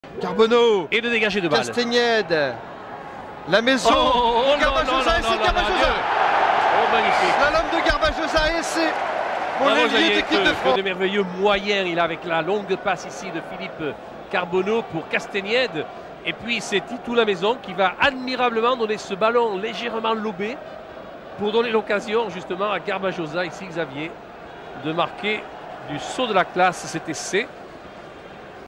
Christophe Lamaison skilful pass creates try
France centre Christophe Lamaison creates a try for France and a first ever international try for wing Xavier Garbajosa against Wales in their 5 Nations match in 1998.